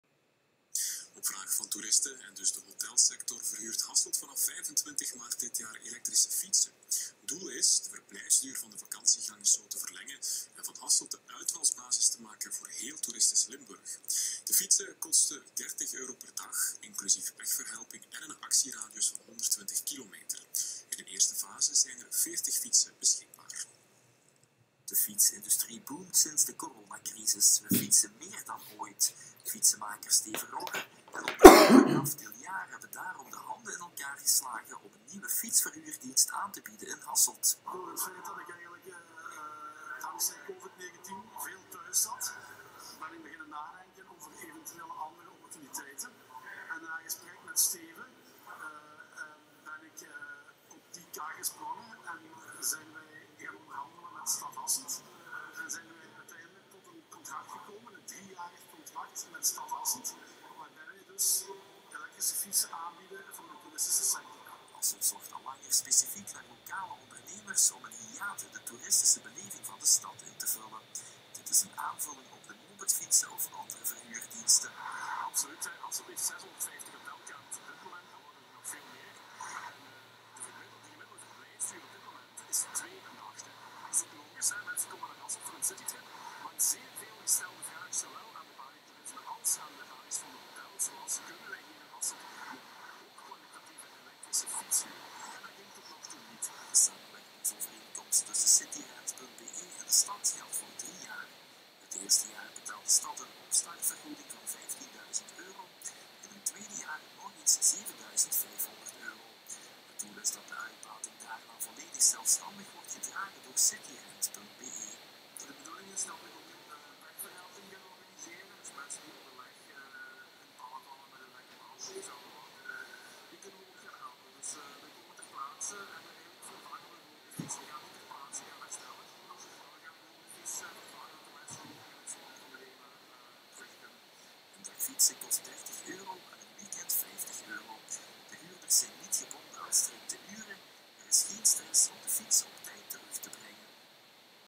City Rent Hasselt bij TV Limburg
De officiële press release van City Rent Hasselt zoals uitgezonden op TV Limburg.